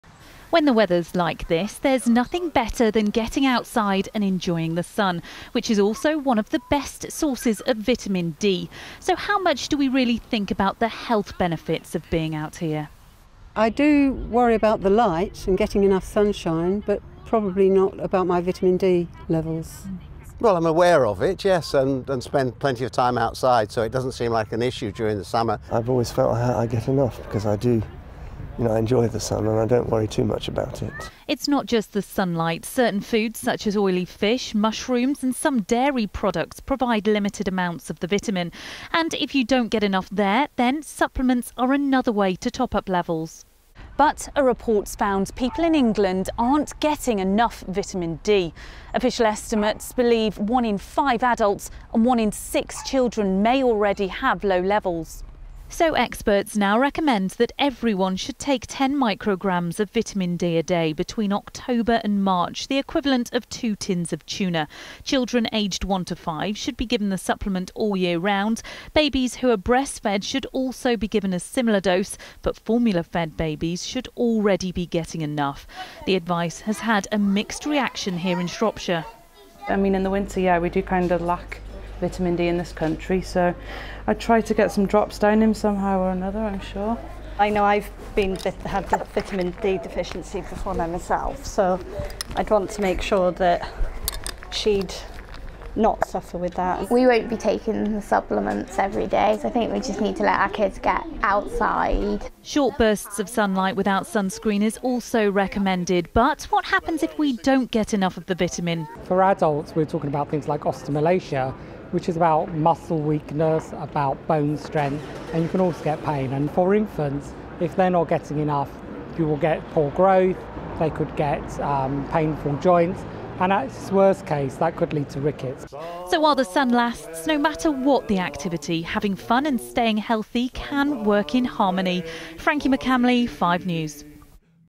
Vitamin D warning: take supplements or risk your health
We should ALL be taking vitamin D supplements in the Autumn and Winter months - that's the latest advice from Public Health England. Levels of vitamin D deficiency in Britain are the highest in 50 years. Experts blame poor diet and people not getting enough sun. It can cause brittle bones - and rickets in children.